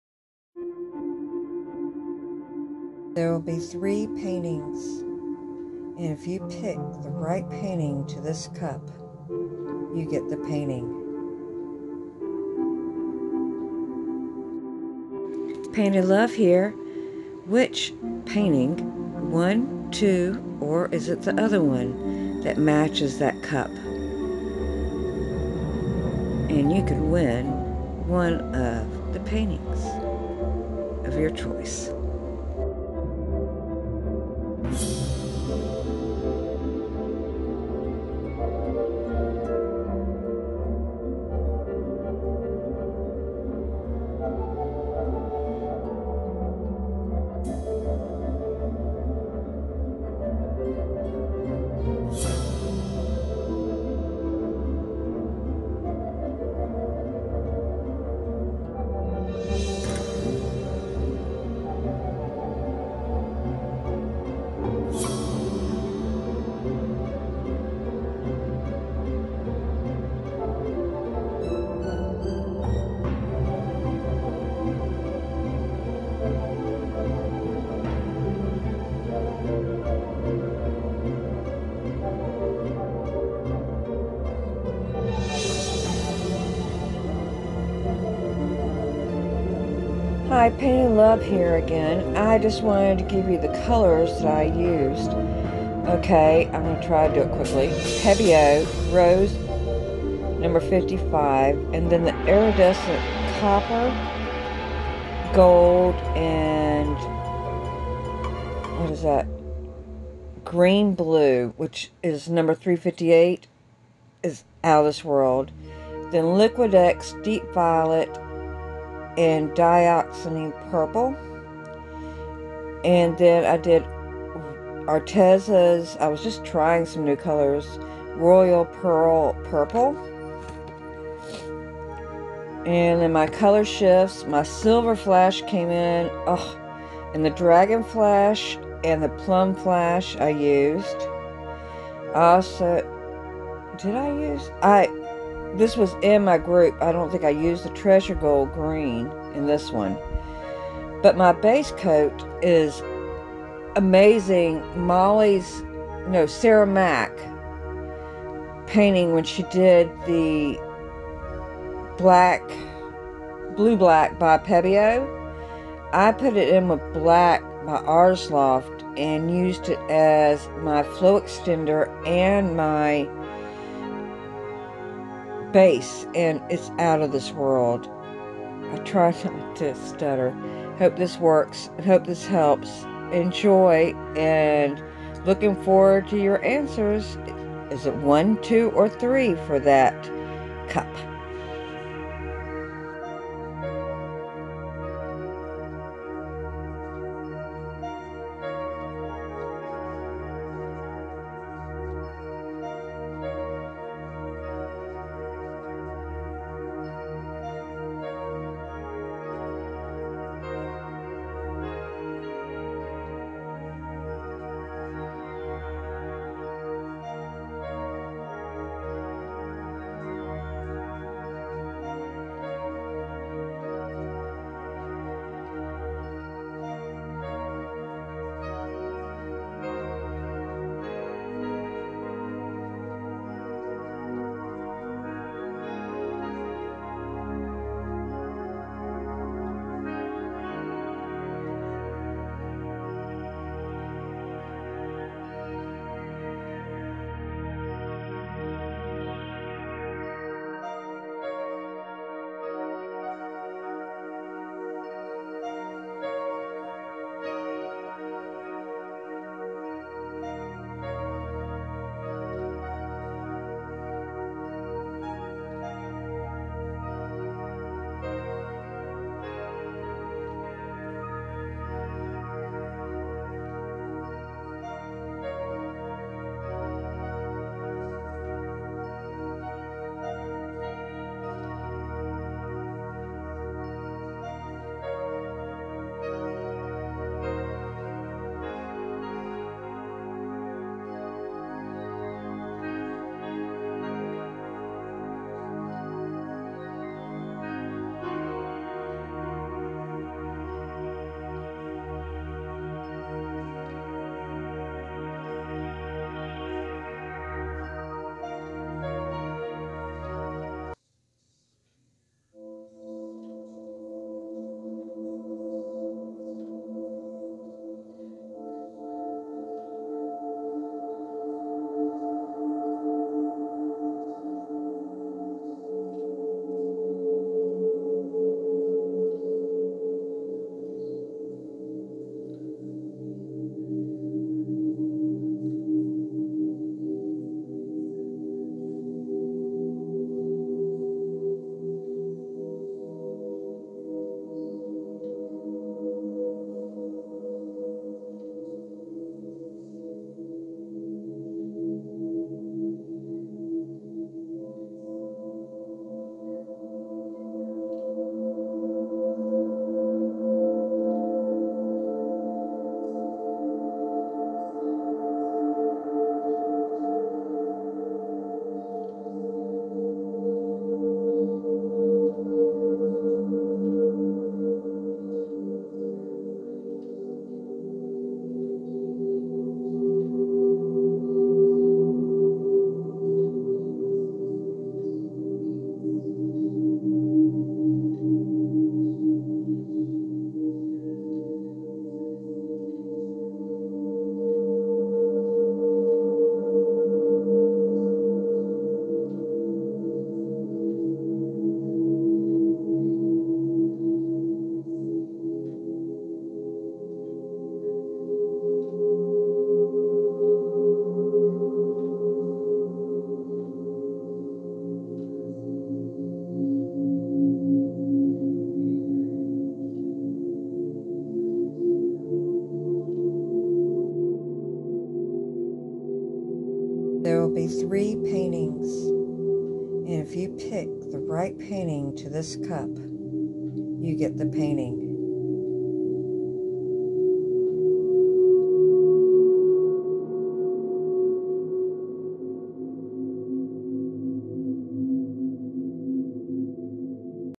confused yet... combo of the 2 for contest
Hey everyone!!
sorry for the confusion. I wanted to put out the other hints before the contest started then it dawned on me. why did you just put it all in one... ding dong..

anyway.. this was fun.
 Rules are simple..
watch , subscribe and like, and share if you want please  and leave a comment either on my facebook page with the picture on it. or on the comments with the video.

This was just a blast and now ..got new stand .. so do some tutorials... yea

HAVE A WONDERFUL FANTASTICAL WEEKEND